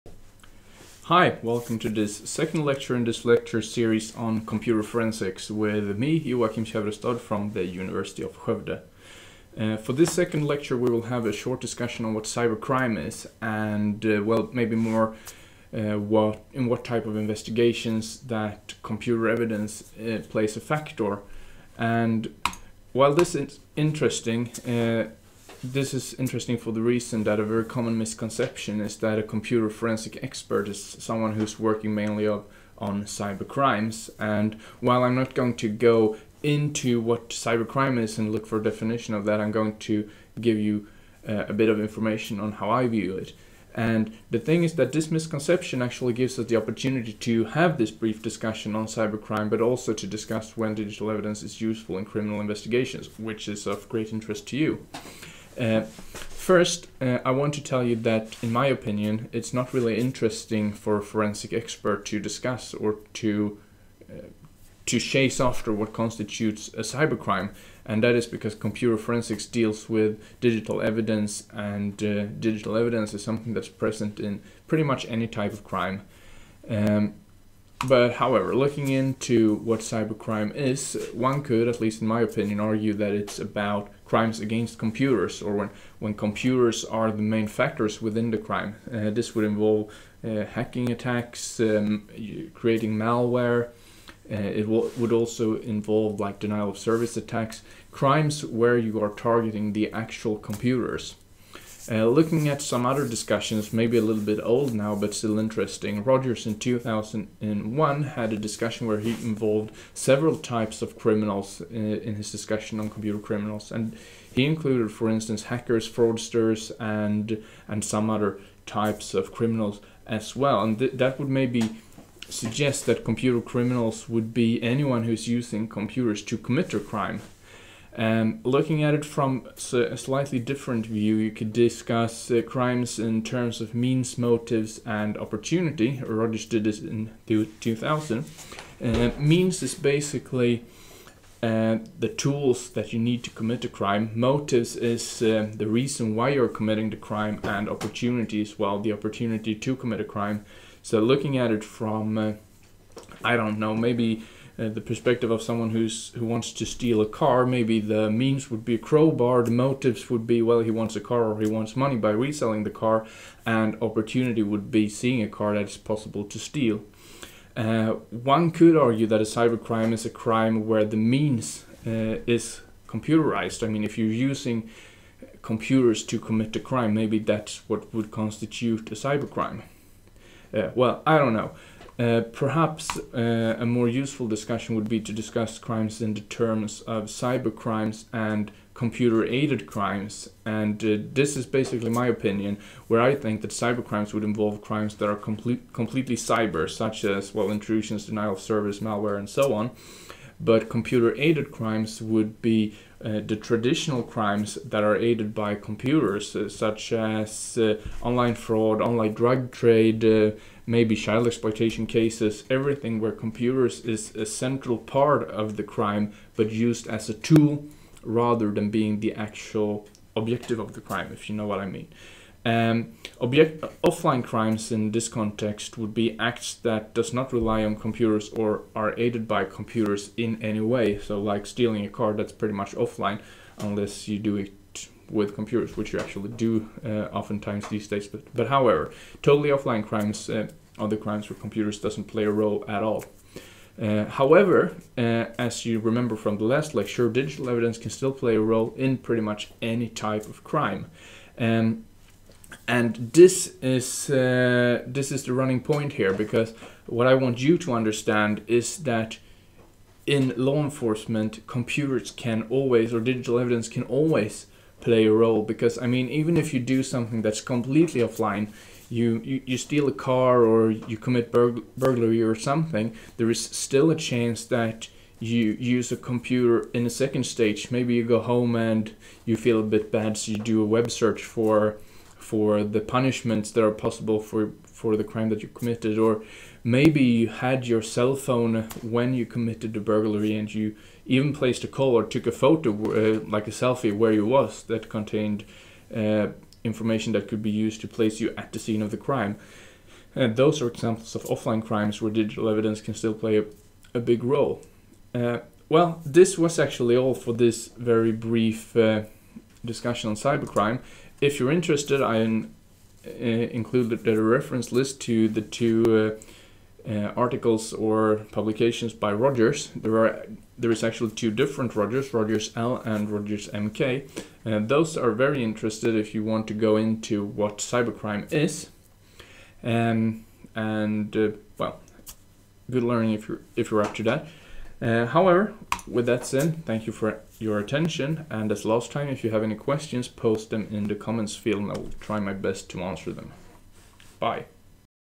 2 - What is cybercrime?
Brief discussion on cybercrime and when digital evidence is interesting in criminal investigations